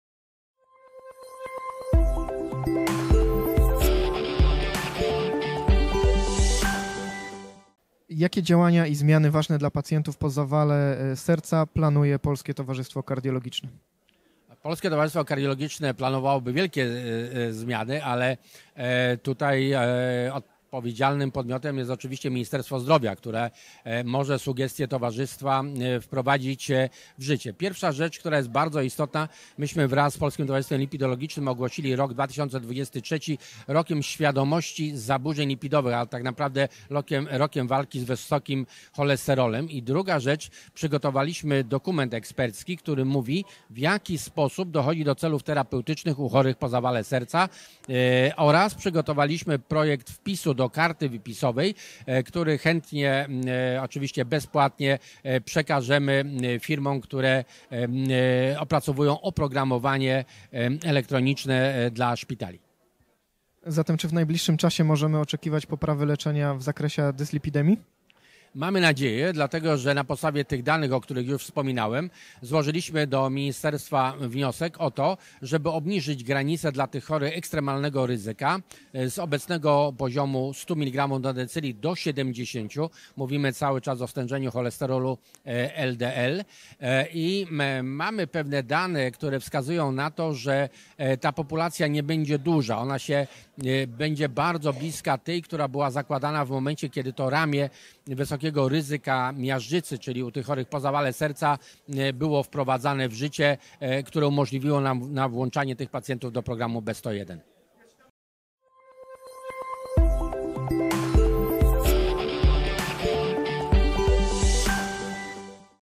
Zawał serca i dyslipidemia – oczekiwania
Jakie zmiany ważne dla pacjentów po zawale serca chciałoby wprowadzić Polskie Towarzystwo Kardiologiczne i czy w najbliższym czasie możemy oczekiwać poprawy leczenia w zakresie dyslipidemii? Na to pytania „Kuriera Medycznego” odpowiedział prof. Przemysław Mitkowski.

Wypowiedź byłego prezesa Polskiego Towarzystwa Kardiologicznego nagraliśmy podczas XXVII Międzynarodowego Kongresu PTK.